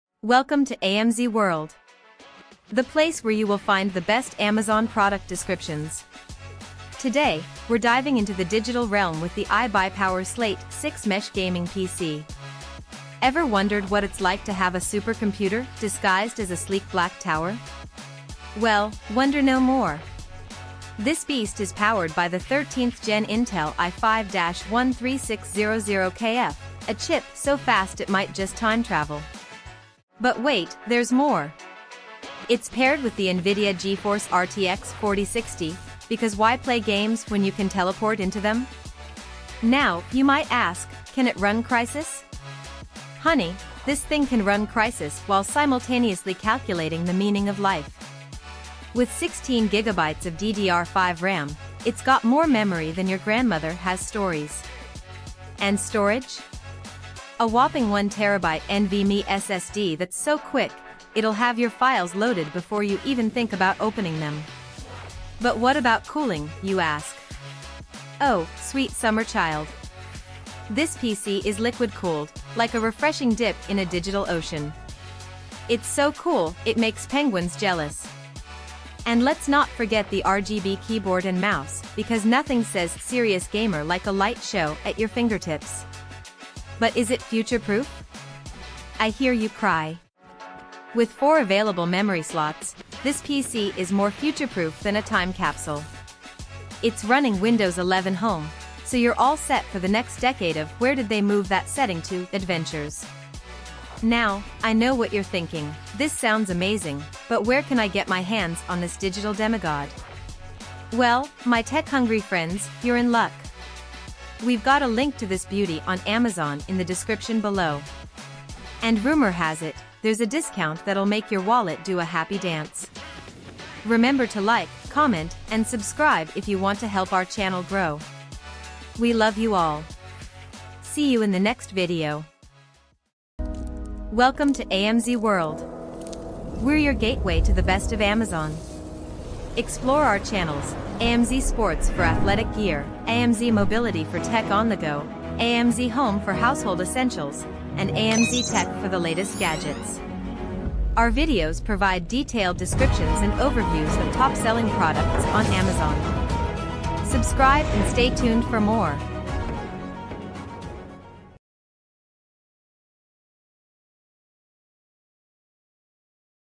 Describing iBUYPOWER Slate 6 Mesh Gaming PC Computer Desktop, Amazon
Here's a breakdown of its key specifications:

Model: iBUYPOWER Slate 6 Mesh Gaming PC
Processor: 13th Gen Intel Core i5-13600KF
Graphics Card: NVIDIA GeForce RTX 4060
RAM: 16GB DDR5
Storage: 1TB NVMe SSD
Cooling: Liquid cooled
Operating System: Windows 11 Home
Accessories: RGB keyboard and mouse included
Color: Black
Form Factor: Computer Tower

Additional details:

CPU Speed: 5.1 GHz (likely the turbo boost speed)
Memory Slots Available: 4
Graphics Card Type: Dedicated

This system is designed for gaming, featuring a recent-generation Intel processor and an NVIDIA RTX 4060 graphics card. The combination of DDR5 RAM, NVMe SSD, and liquid cooling suggests it's built for performance.
Would you like me to elaborate on any specific component or aspect of this computer?

Today, we're diving into the digital realm with the iBUYPOWER Slate 6 Mesh Gaming PC. Ever wondered what it's like to have a supercomputer disguised as a sleek black tower? Well, wonder no more! This beast is powered by the 13th Gen Intel i5-13600KF, a chip so fast it might just time travel. But wait, there's more! It's paired with the NVIDIA GeForce RTX 4060, because why play games when you can teleport into them?

Now, you might ask, "Can it run Crysis?" Honey, this thing can run Crysis while simultaneously calculating the meaning of life! With 16GB of DDR5 RAM, it's got more memory than your grandmother has stories. And storage? A whopping 1TB NVMe SSD that's so quick, it'll have your files loaded before you even think about opening them.

But what about cooling, you ask? Oh, sweet summer child. This PC is liquid-cooled, like a refreshing dip in a digital ocean. It's so cool, it makes penguins jealous. And let's not forget the RGB keyboard and mouse - because nothing says "serious gamer" like a light show at your fingertips.

"But is it future-proof?" I hear you cry. With 4 available memory slots, this PC is more future-proof than a time capsule. It's running Windows 11 Home, so you're all set for the next decade of "Where did they move that setting to?" adventures.

Now, I know what you're thinking: "This sounds amazing, but where can I get my hands on this digital demigod?" Well, my tech-hungry friends, you're in luck! We've got a link to this beauty on Amazon in the description below. And rumor has it, there's a discount that'll make your wallet do a happy dance.